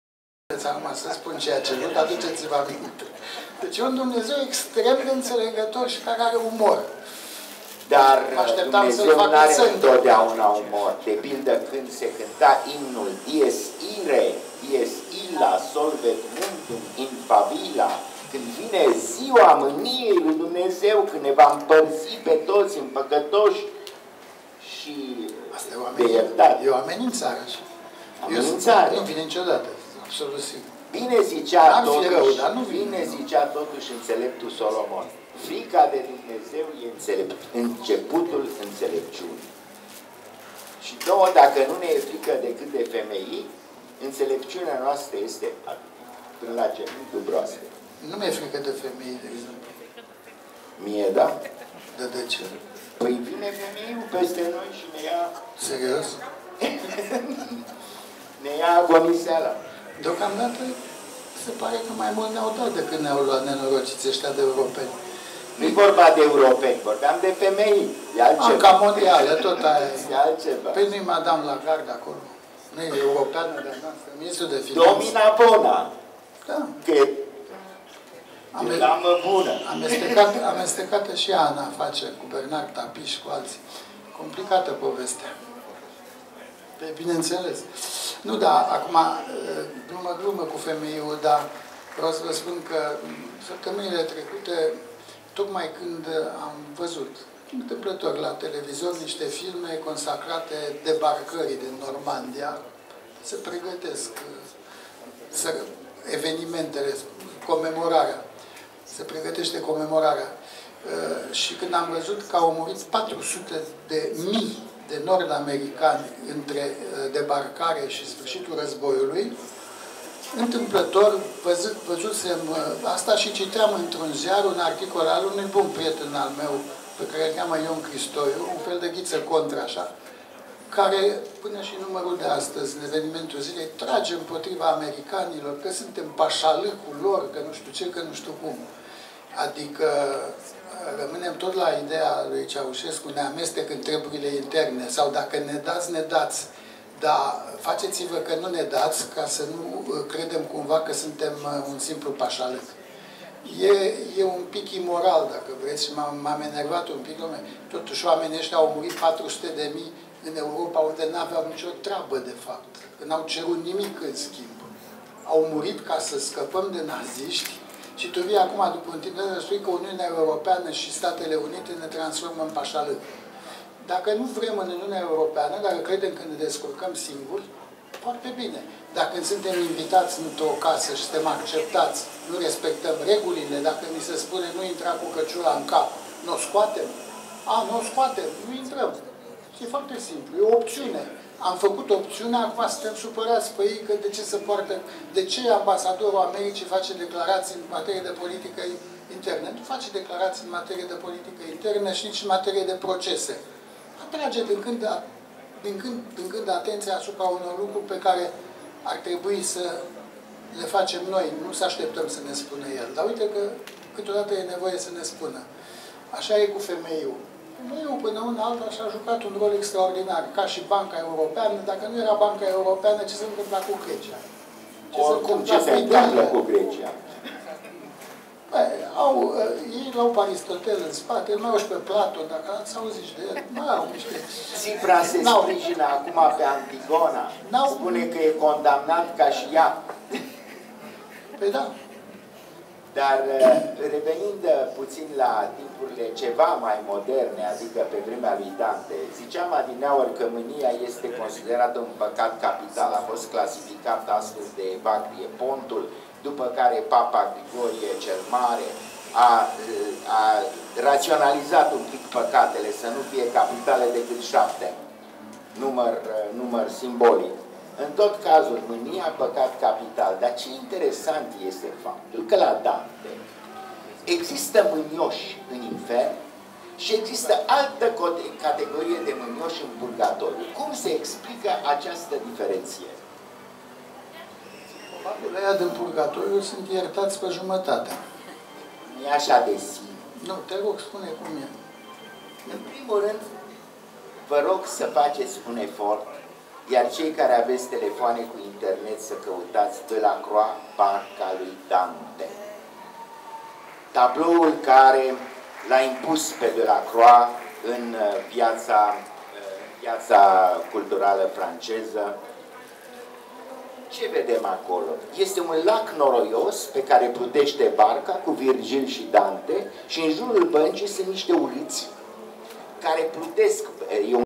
Nicolae Manolescu și Adrian Mihalache -Editura ICR- BOOKFEST București-4 06 2016-v3 Adriana Weimer
Salonul Internațional de Carte BOOKFEST București - ediția a XI-a - la Romexpo - 4-5 iunie 2016 -video 1-28 by Adriana Weimer